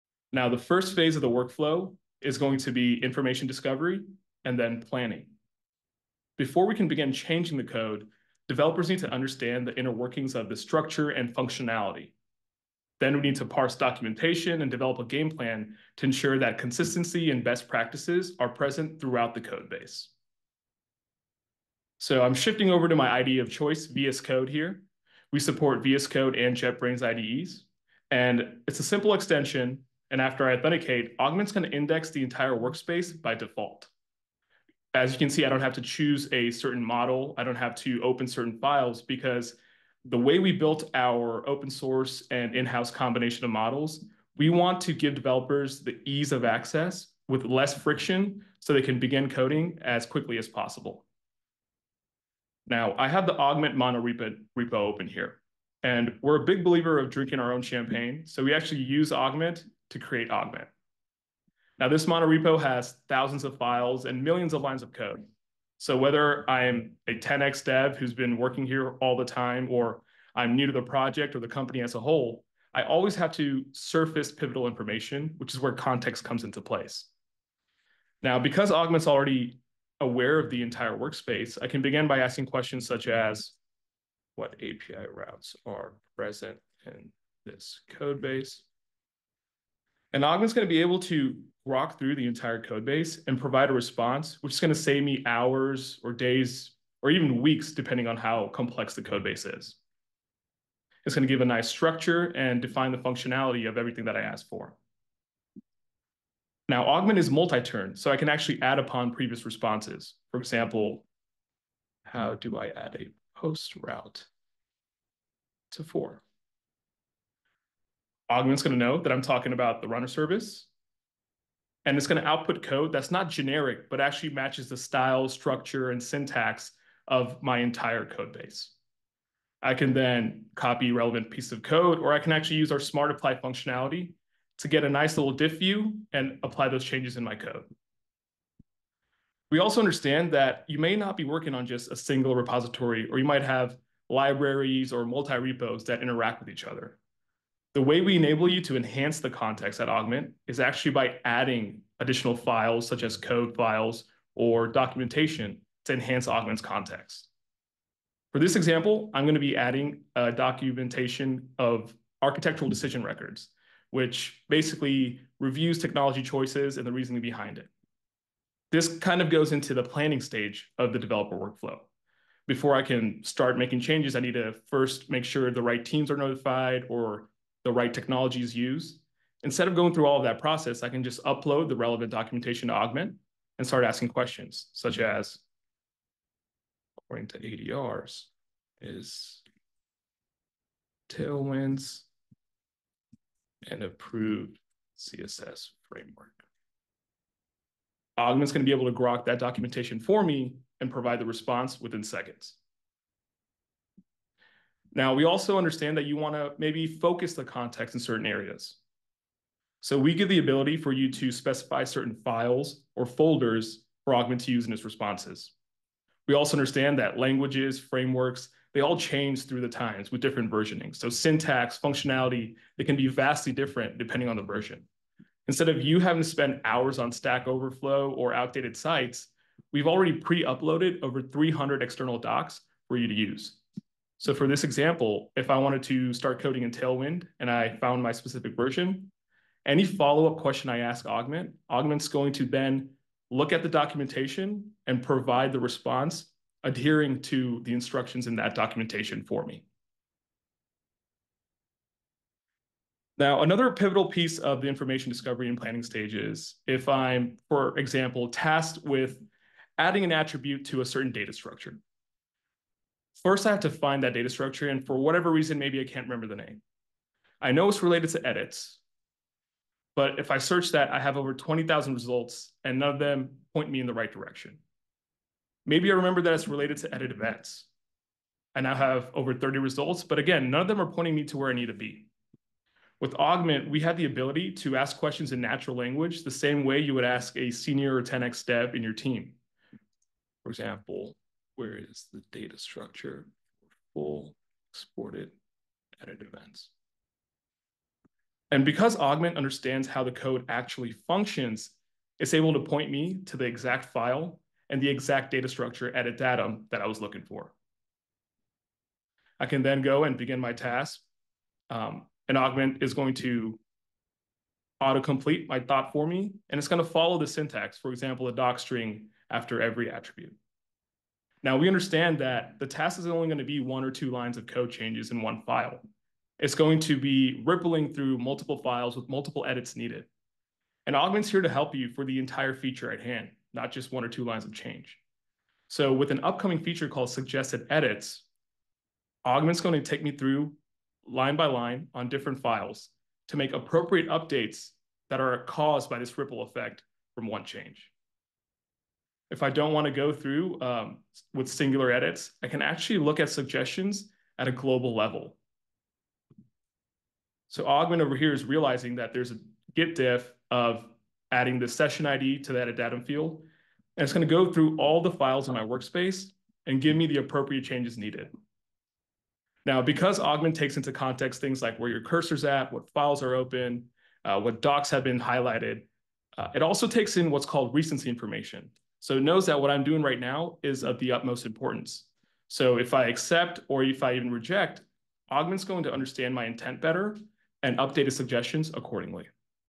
Developer Journey Pt. 1: Getting up to speed on complex projects quickly
Before developers begin changing code, they need to understand the inner workings of the code's structure and functionality. They must then review documentation and develop a game plan to ensure consistency and adherence to best practices throughout the codebase.

In this demo, Solutions Engineer Anshuman demonstrates how Augment can help developers quickly get up to speed on complex projects—using Augment’s very own monorepo as an example.